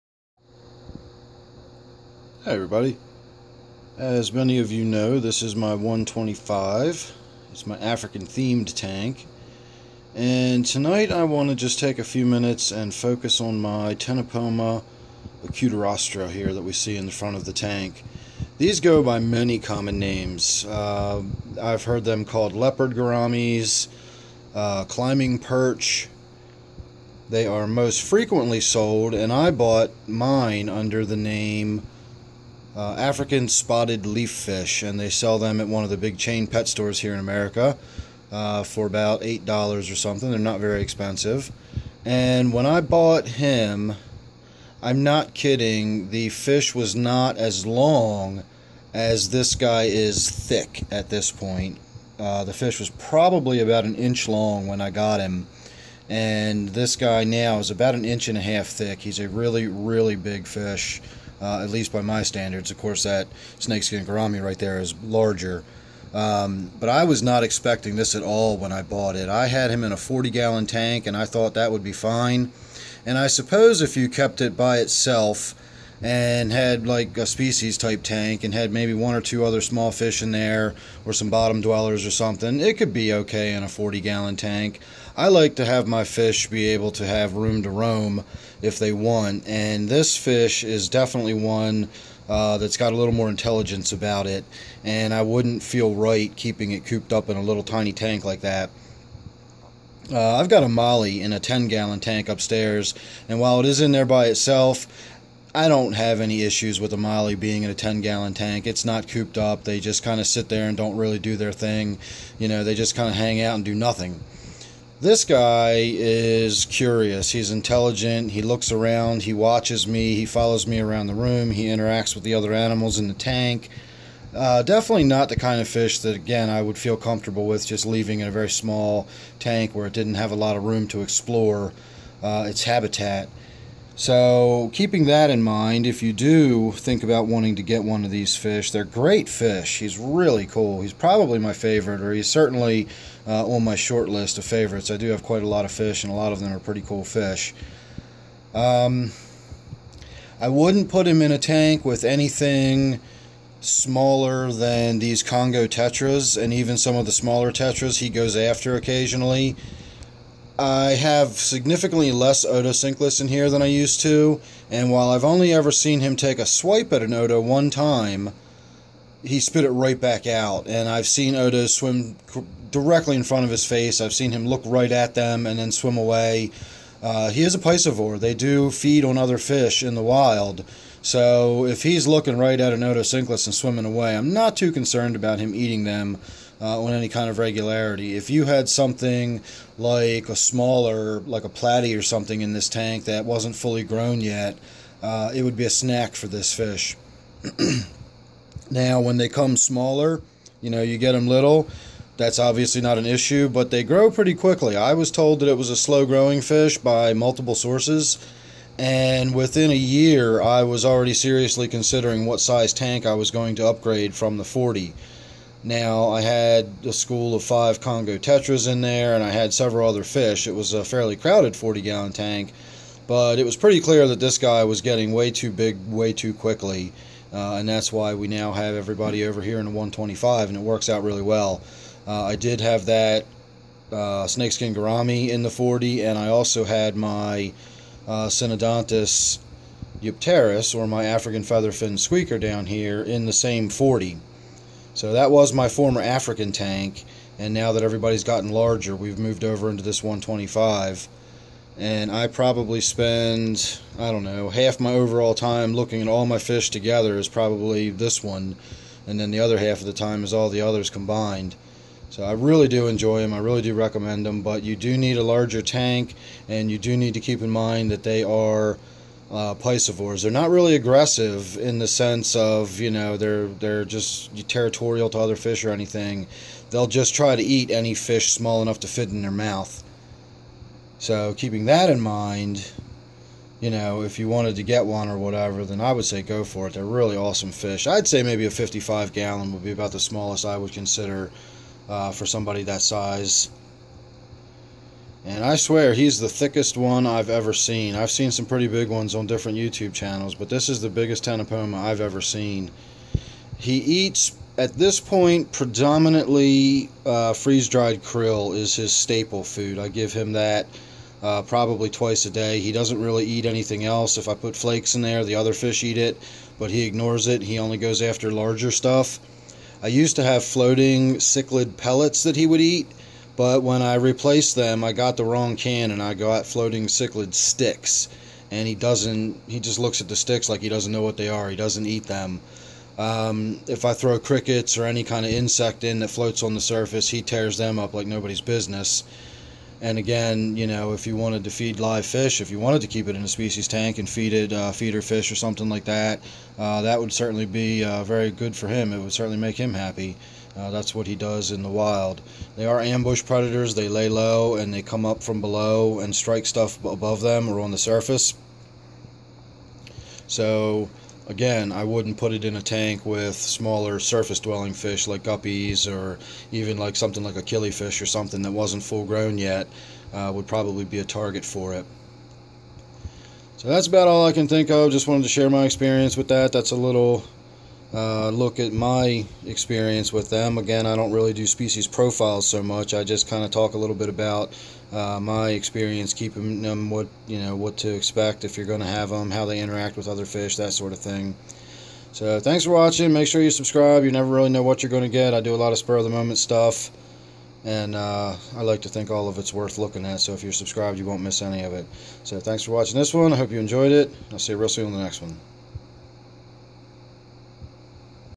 Spotted African Leaf Fish or Leopard Gourami (Ctenopoma Actirostre)
Just a look at my Leopard Ctenopoma, which has many common names, including: Climbing Perch, African Spotted Leaf Fish, Spotted African Leaf Fish, Spotted Gourami, Leopard Gourami, Bush Gourami, African Bush Fish Leopard Bush Fish, and others. I also have a Snakeskin gourami, Syanodontis Eupterus (African Featherfin squeaker Catfish), Clown Loaches, Congo Tetras, Otocinclus Catfish (Otos) and more.
This is a 125 gallon, freshwater planted tank. Tropical Aquarium.